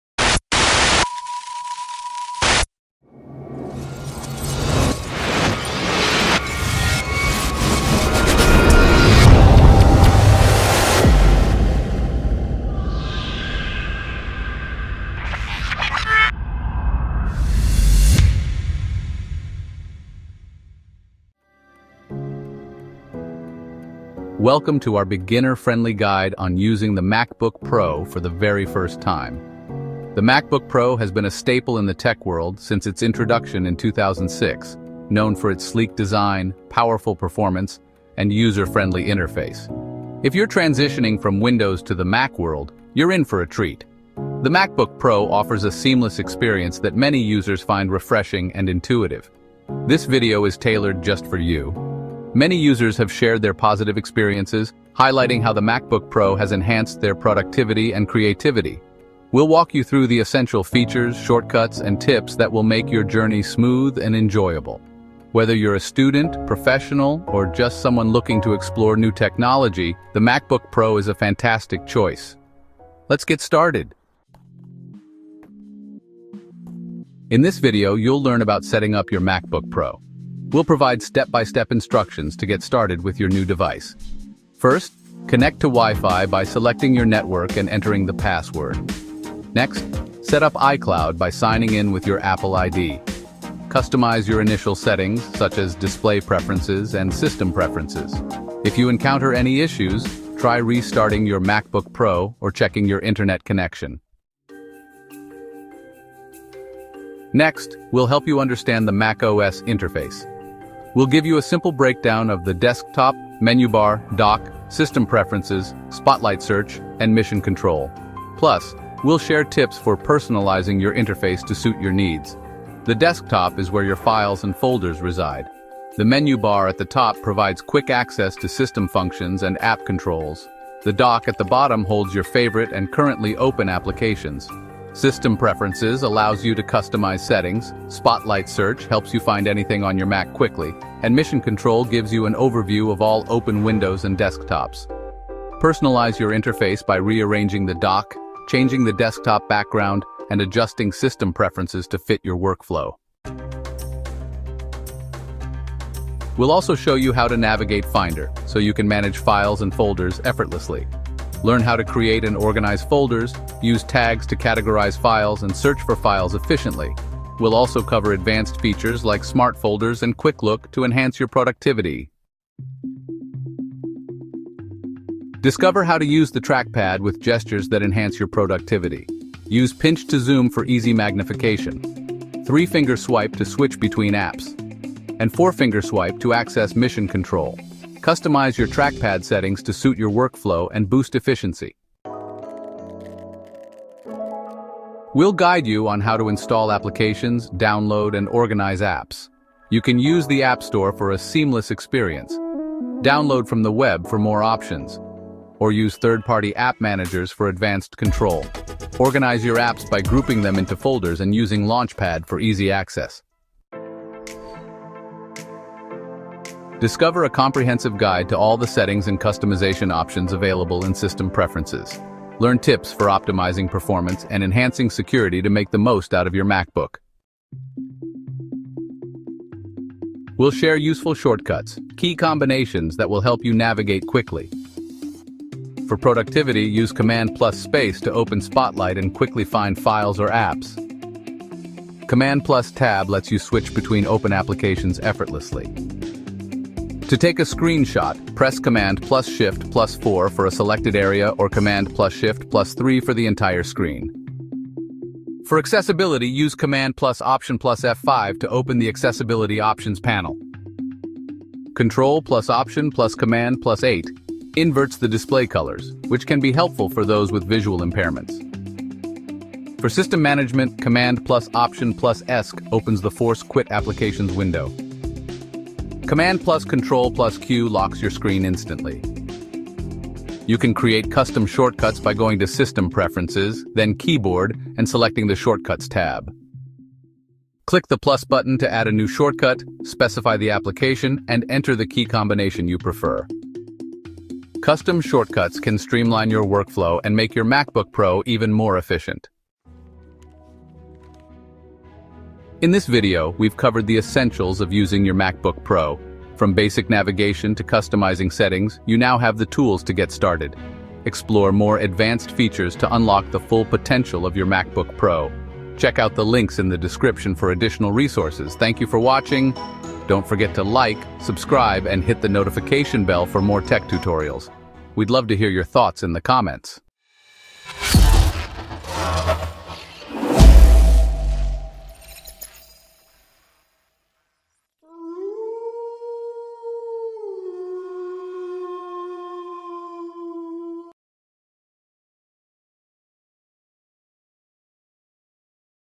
Mastering Your MacBook Pro: A Beginner's Step-by-Step Guide
Welcome to "Mastering Your MacBook Pro: A Beginner's Step-by-Step Guide"! 🌟 In this comprehensive video, we’ll take you through everything you need to know to get started with your MacBook Pro. From setting up your device and navigating the macOS interface to utilizing essential applications and customizing your settings, this guide is perfect for new users looking to unlock the full potential of their Mac.

- Initial setup and configuration
- Tips for navigating the macOS interface
- Key features of the MacBook Pro
- Essential apps for productivity and creativity
- Customization options to enhance your experience

Whether you're transitioning from another operating system or diving into the world of Macs for the first time, this video will equip you with the knowledge and confidence to use your MacBook Pro like a pro!

Thank you to our amazing supporters for making this content possible! Your encouragement inspires us to create valuable resources for our community. 💻✨